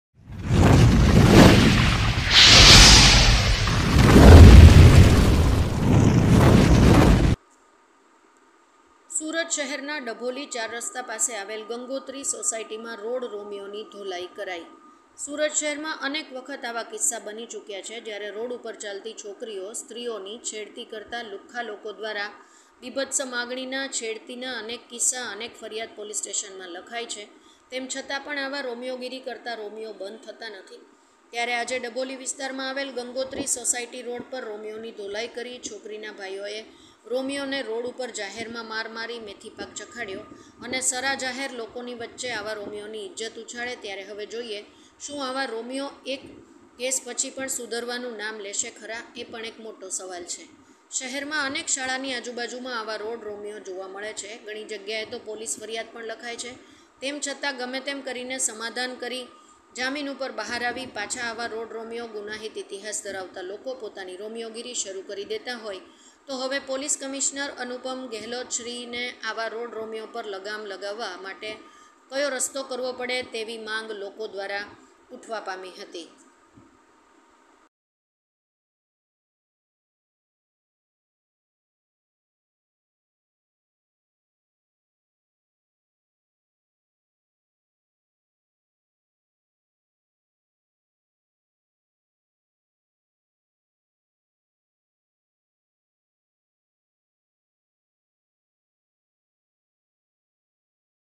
NEWS સુરત શહેરના ડભોલી ચાર રસ્તા પાસે આવેલ ગંગોત્રી સોસાયટીમાં રોડ રોમિયોની ધોળાઈ કરાઈ...
સુરત શહેરના ડભોલી ચાર રસ્તા પાસે આવેલ ગંગોત્રી સોસાયટીમાં રોડ રોમિયોની ધોળાઈ કરાઈ...

સુરત શહેરમાં અનેક વખત આવા કિસ્સા બની ચૂક્યા છે જ્યારે રોડ ઉપર ચાલતી છોકરીઓ અને બાયોને છેડતી કરતા લુખ્ખા લોકો દ્વારા છેડતી બીપસ્ત માંગણી ના અનેક કિસ્સા અને અનેક ફરિયાદ પણ પોલીસ સ્ટેશનમાં લખાય છે તેમ છતાં પણ આવા રોમિયોગીરી કરતા રોમિયો બંધ નથી થતા ત્યારે આજે ડભોલી વિસ્તારમાં આવેલ ગંગોત્રી સોસાયટી રોડ પર રોમિયોની ધોળાઈ કરી અને છોકરીના ભાઈઓએ રોમિયો ને રોડ ઉપર જાહેરમાં માર મારી મેથીપાક જખડ્યો અને સરા જાહેર લોકોની વચ્ચે આવા રોમિયોની ઈજ્જત ઉછાળે ત્યારે હવે જોઈએ શું આવા રોમિયો આ એક કેસા પછી પણ સુધારવાનું નામ લેશે ખરા એ પણ મોટો સવાલ છે.. ?

શહેરમાં અનેક શાળાની આજુબાજુમાં આવા રોડ રોમિયો જોવા મળે છે ઘણી જગ્યાએ તો પોલીસ ફરિયાદ પણ લખાય છે તેમ છતાં પણ ગમે એમ કરીને સમાધાન કરી અથવા તો જામીન ઉપર બહાર આવી પાછા આવા રોડ રોમિયો ગુનાહિત ઇતિહાસ ધરાવતા લોકો પોતાની રોમિયોગીરી શરૂ કરી દેતા હોય તો હવે પોલીસ કમિશનર અનુપમ શ્રી ગેલોત સાહેબને આવા રોડ રોમિયો ઉપર લગામ લગાવવા માટે કઈ રસ્તો કરવો પડે તેવી માંગ લોકો દ્વારા ઉઠવા પામી હતી.